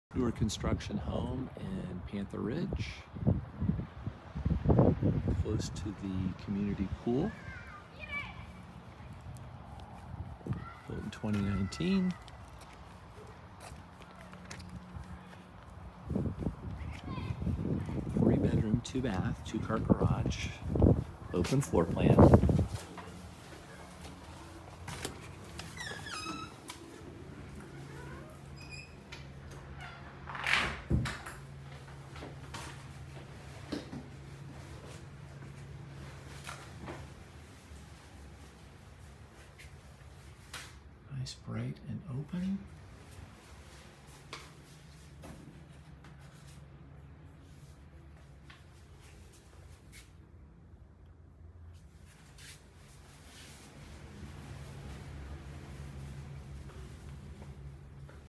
Price Reduced 2019 Built Home at 376 Snook Place , Panther Ridge Cocoa Fl. Great Central location!!!
2019 BUILD SHORT DRIVE TO : BEACHES, SPACE X , BOLUE ORIGINS

PRICE REDUCED BREVARD COUNTY

FULL PREVIEW VIDEO

PROPERTY LISTING